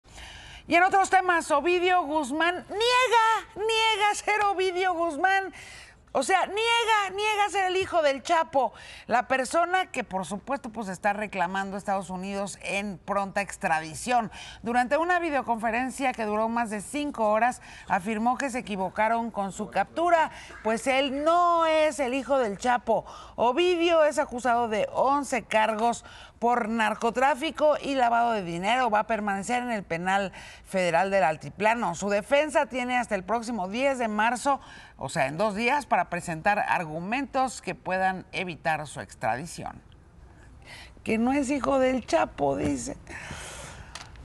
Ovidio Guzmán niega su identidad | Noticias con Yuriria Sierra
"No soy la persona que reclama Estados Unidos". OvidioGuzmán, hijo de ElChapo Guzmán, niega su identidad.